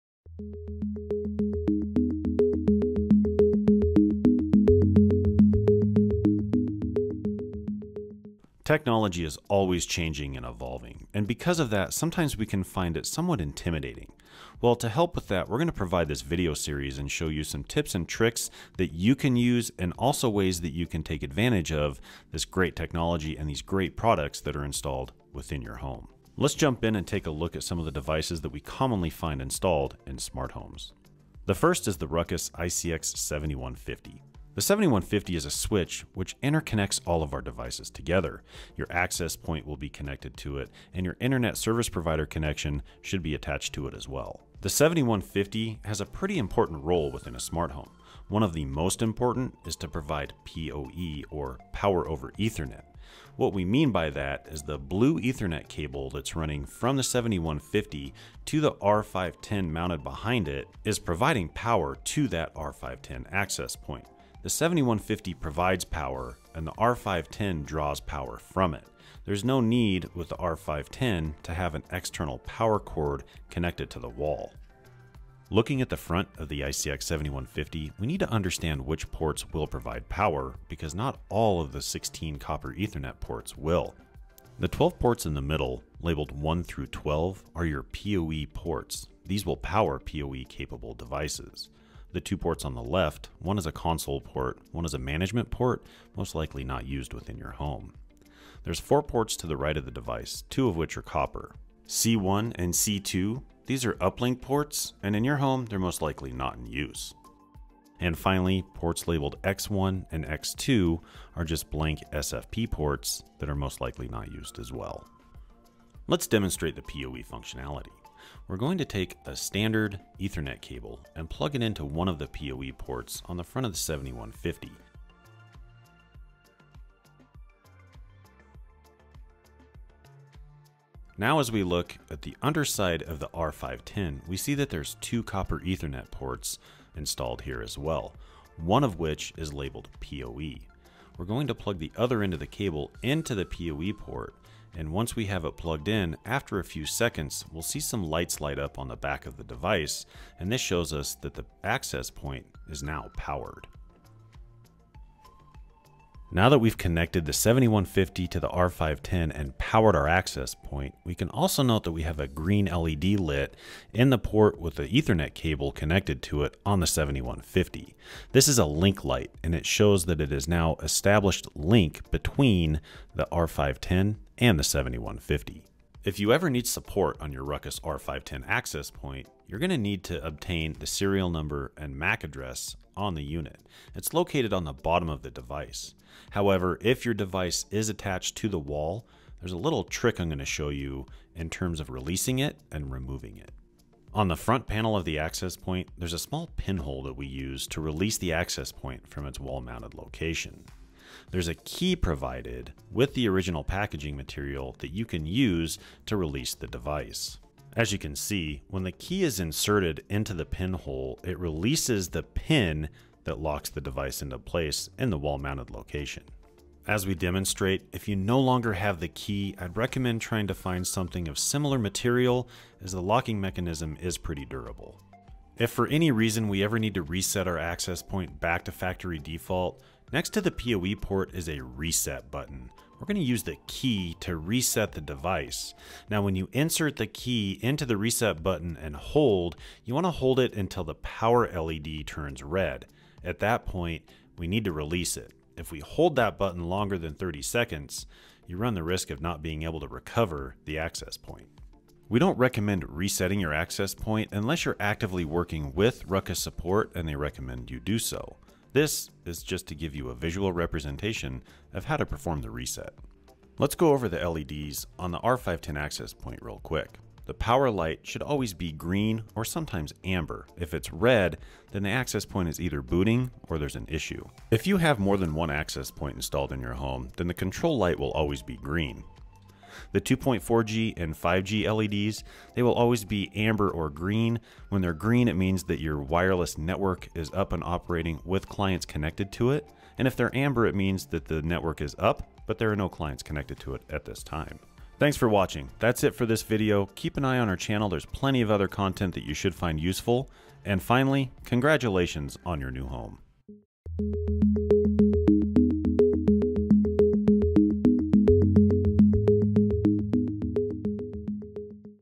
Ruckus at Home: Introduction
If you're watching this video I'd like to inform you that on Wednesday July 15th at 11AM MST we are performing a Live Stream right here on YouTube to help give you more information on your RUCKUS at Home products.  Swing by the live stream, say hi and ask questions.  We will be there to help!

Provides an introduction to Ruckus devices that are commonly installed in a smart home, the ICX 7150 Switch and the R510 Access Point (AP). Learn to identify the ports, what the LEDs mean, and how to remove your AP from its wall mount.

For more information about how to use and troubleshoot Ruckus devices that are installed in a home, take a look at the Ruckus at Home page on the How-To Hub at: